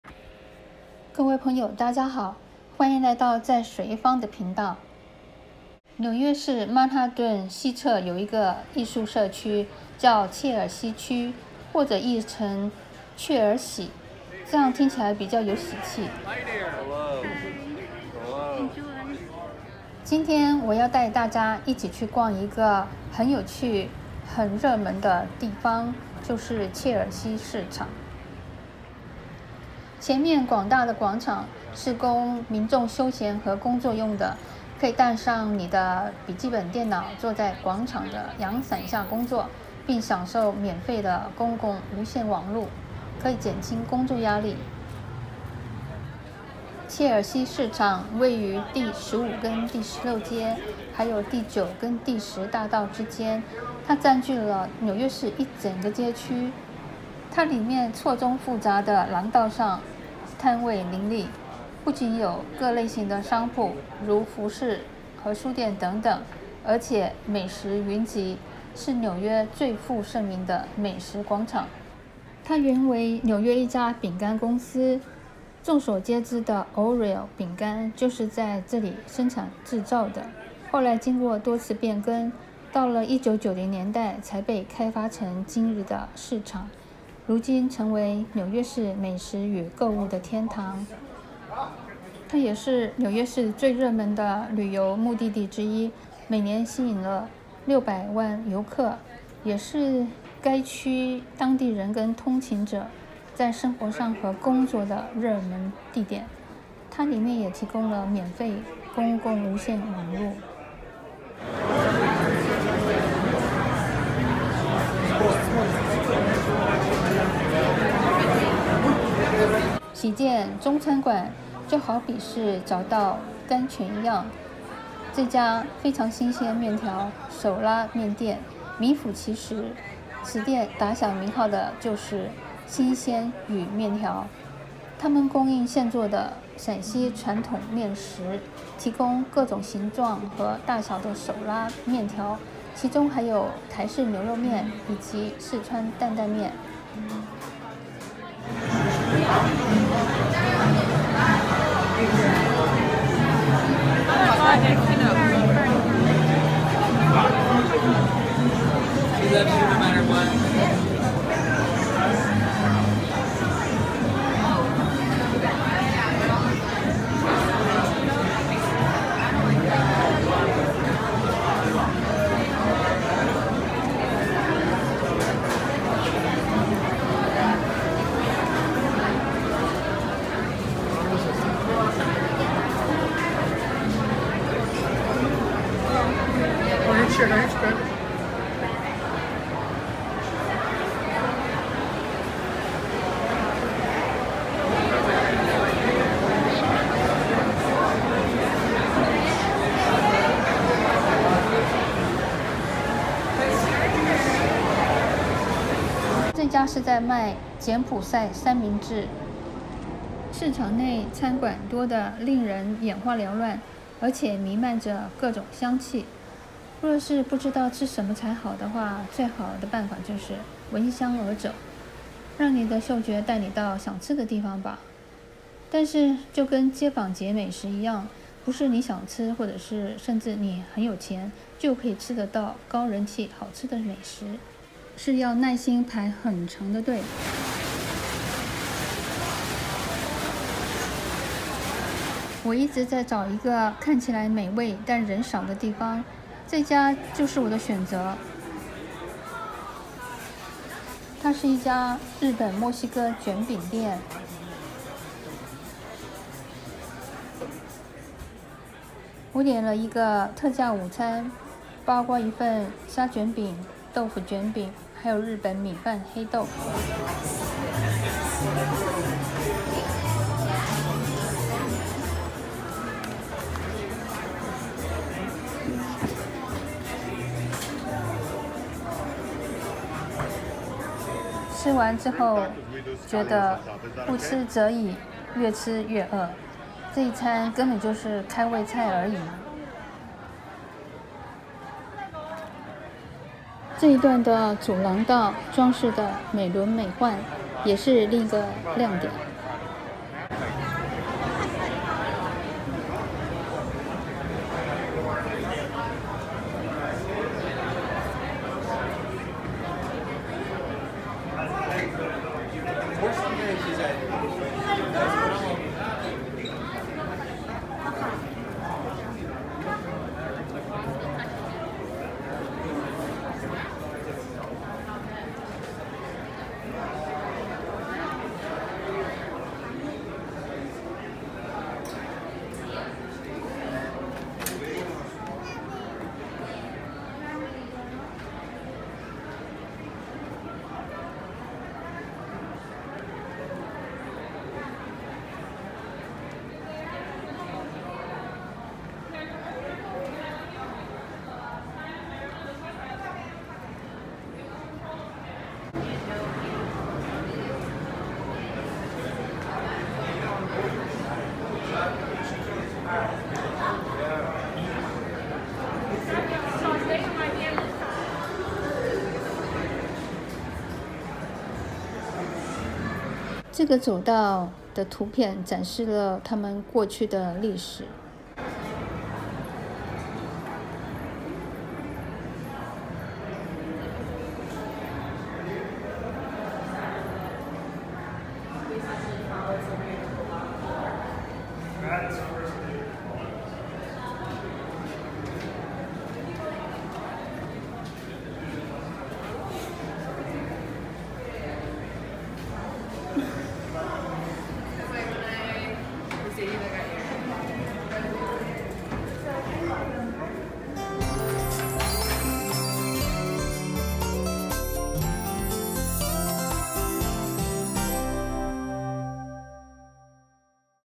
紐約生活紀實｜曼哈頓, 巨型的美食與購物天堂: 從舊廠房, 華麗轉身為超高人氣、新潮的切爾西市場; 陝西傳統麵食, 發光發亮; 日本墨西哥捲餅, 不吃則已, 越吃越餓!
曼哈頓, 巨型的美食與購物天堂: 從舊廠房, 華麗轉身為超高人氣、新潮的切爾西市場; 陝西傳統麵食, 發光發亮; 日本墨西哥捲餅, 不吃則已, 越吃越餓!